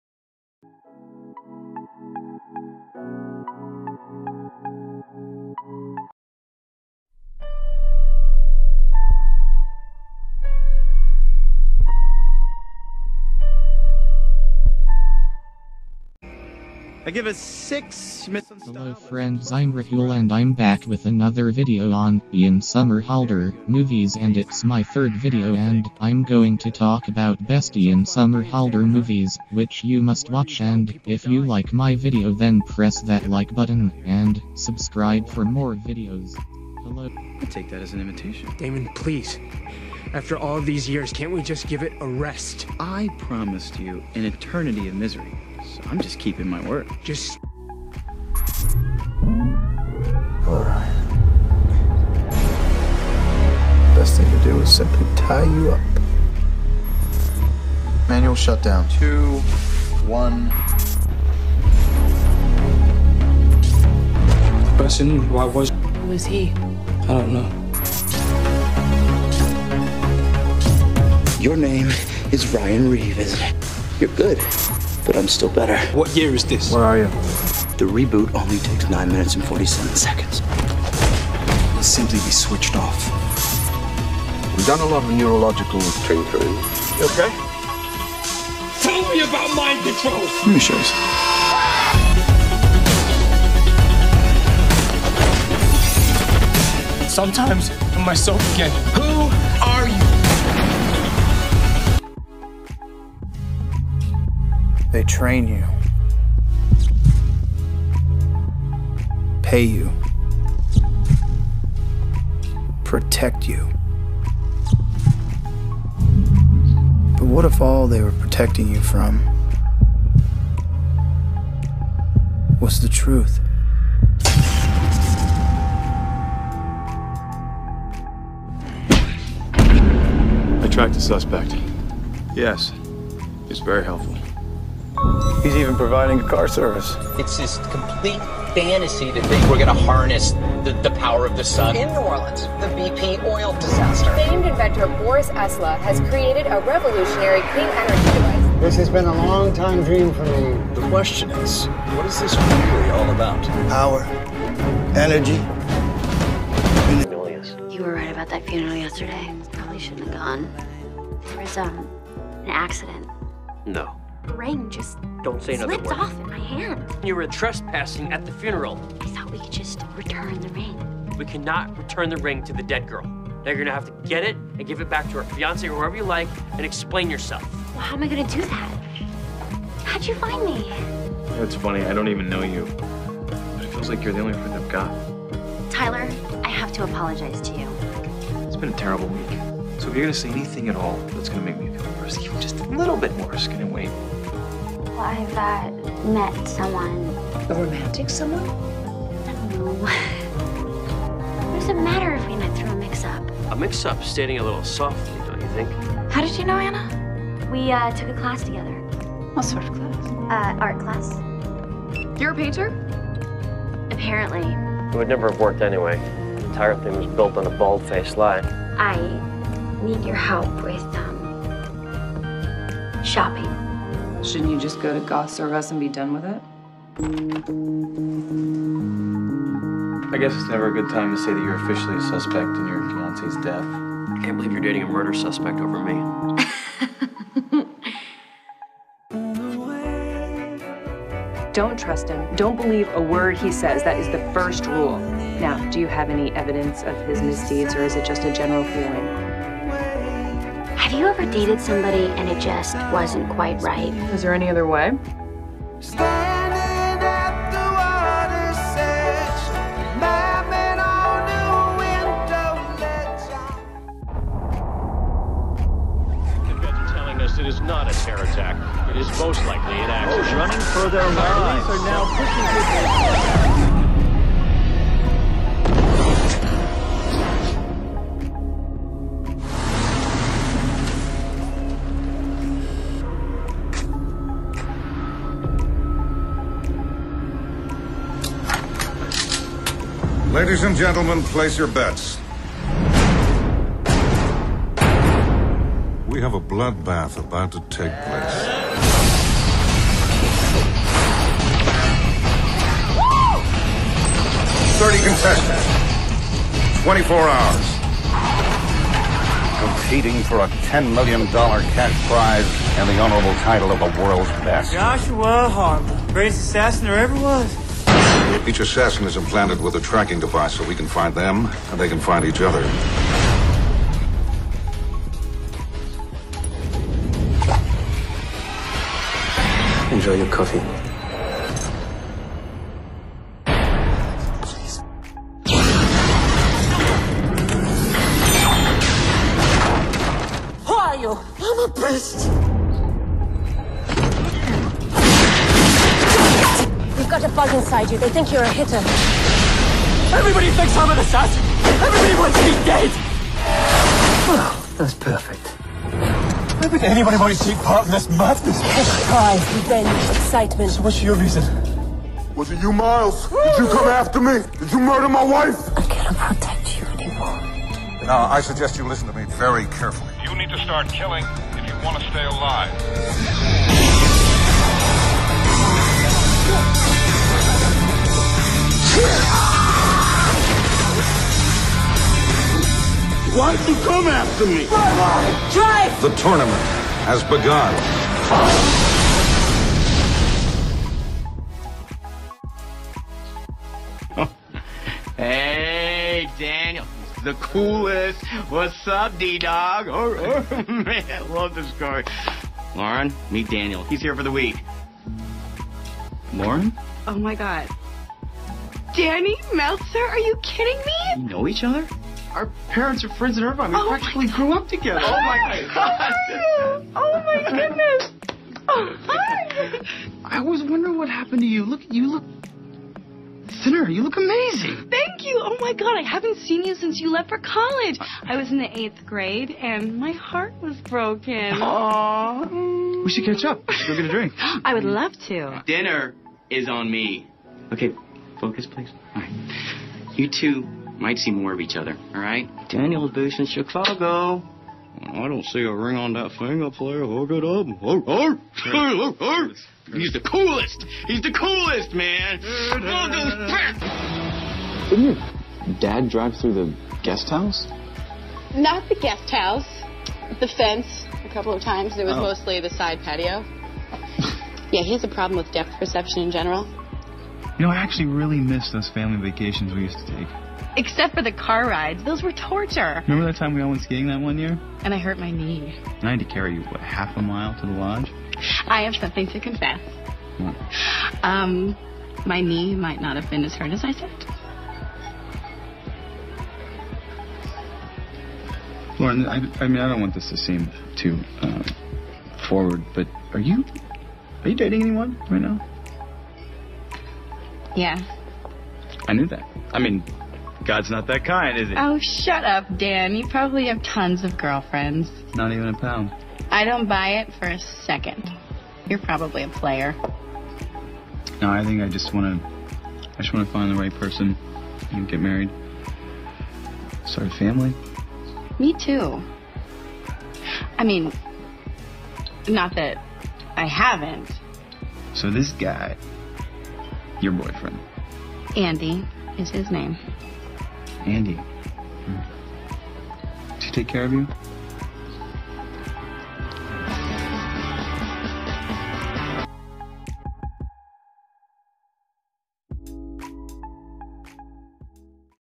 best Ian Somerhalder Movies
Hello Everyone! Welcome, To Vampyre Flix And Today I'm going to talk about Best Ian Somerhalder movies and He had a great contribution to the cinema industry and please like my video and subscribe to my channel.

1. Anomaly

2. Time Framed

3. Tournament

4. Wake

5. How To Make Love To A Woman